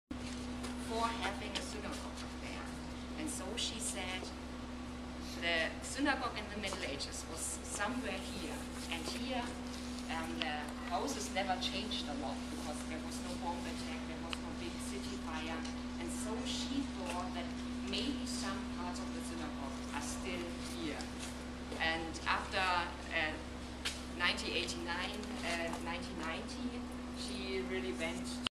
Guided tour Old Synagogue part [1/11]
EUROCLIO Annual Conference, Erfurt, Germany (2013)